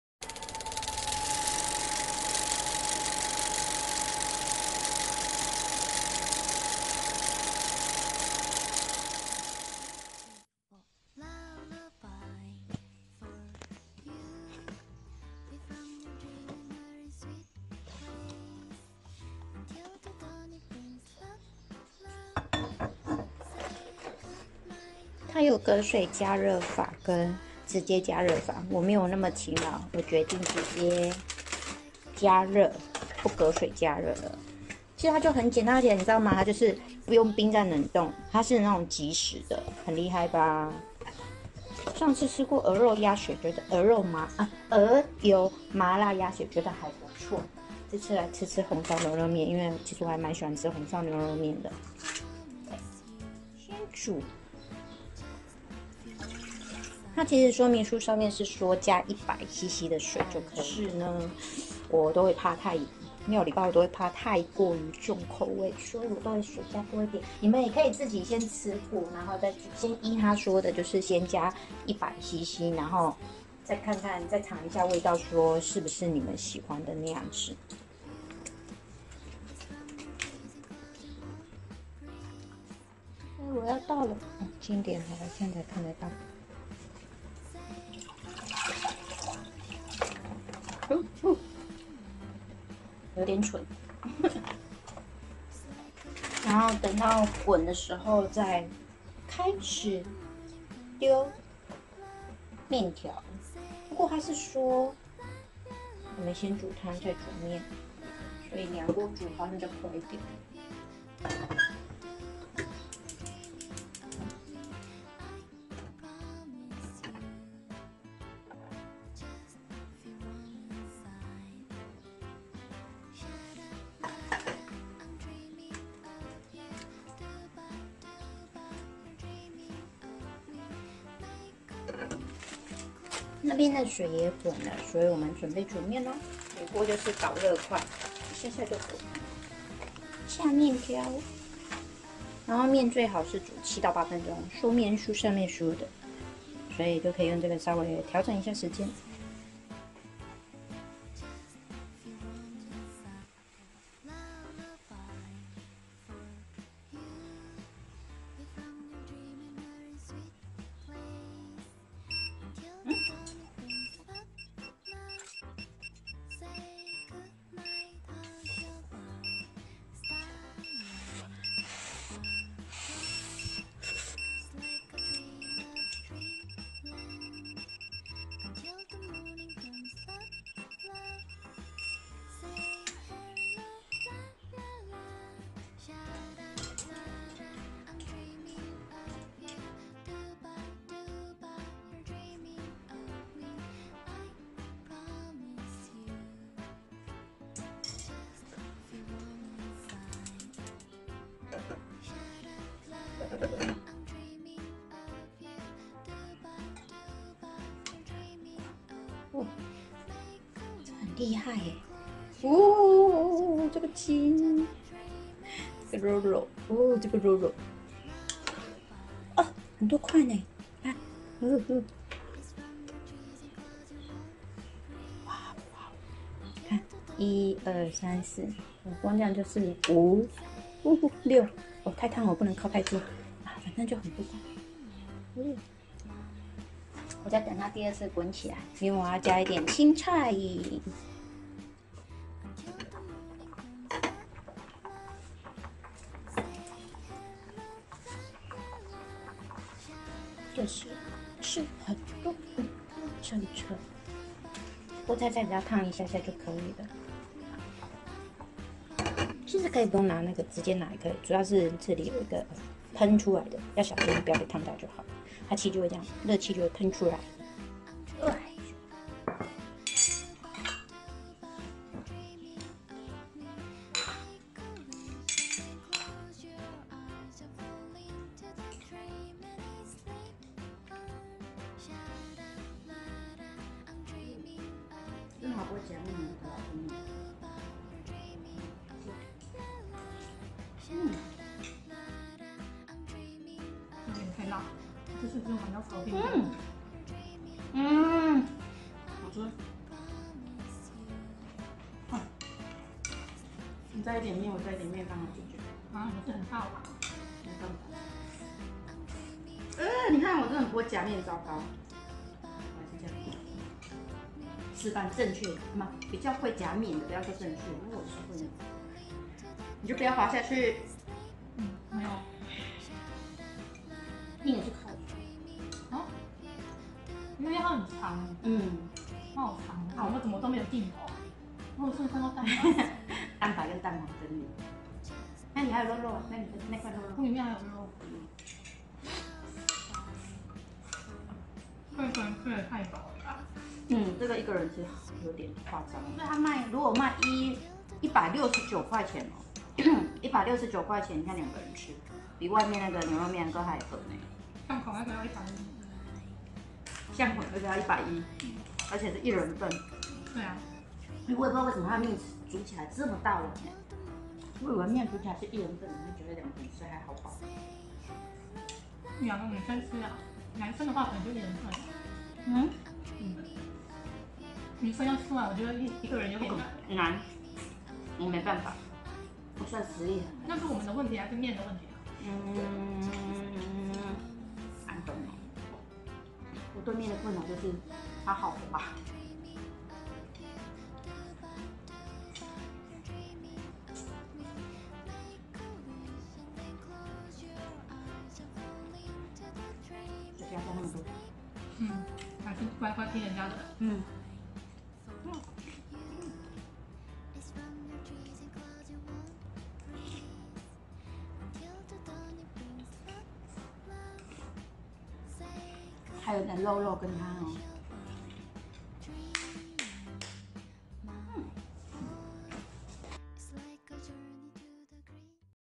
【Vlog】醫生健康日記輕鬆15分鐘端出美味紅燒牛肉麵(酒窩小姊sunny 自煮日常)
今晚，我想要來碗“紅燒牛肉麵”
醫生健康日記，簡單料理
輕鬆15分鐘如同現煮端出好菜！

😗「我覺得...大家好像發現了...」
😶『發現什麼？』
😗「我覺得大家好像發現了我們家很愛吃“牛肉麵”」
😏『都說了可以整理一篇－我吃過的牛肉麵之宅配團購美食系列 了。』

🤫偷偷說...
我覺得醫生健康日記的鵝油鴨血超好吃(附冬粉)
然後，鵝油麻辣鴨血原價280元 團購價只要129元
喜歡麻辣鴨血的朋友
別忘了順手放進購物車喲～